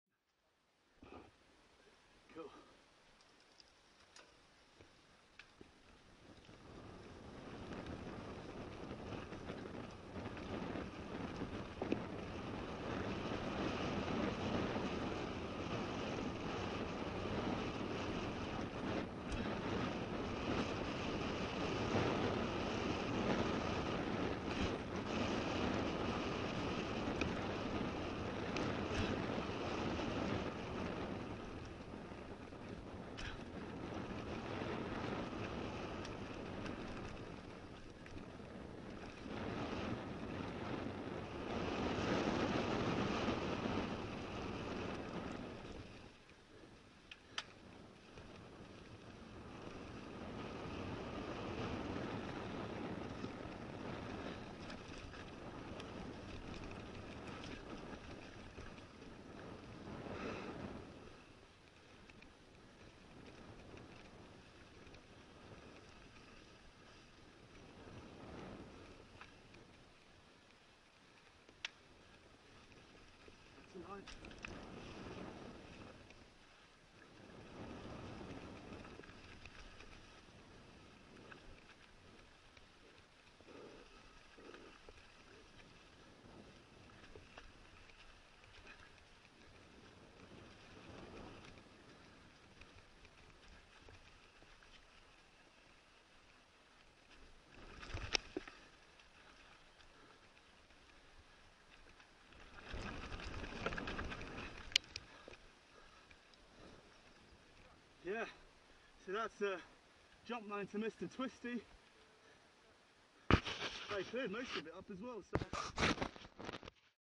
Sloppy Forest of Dean - MTB - Jump Line to Mr Twisty - Nukeproof Mega 290 Vs Yeti SB165
Ripping it down Jump Line to Mr Twisty. If you like your tech and you like it moderately steep with features, Jump Line to Mr Twisty comprises of 4 fast pops which shoot you into a sequence of flat turns, before spitting you into 2 roll-ins. The wet mulch I would say demands something like a set of mud tyres. I had my Michelin Wild Enduros whilst Adam​ was running Wild Muds, which seem more apt for FoD this time of year.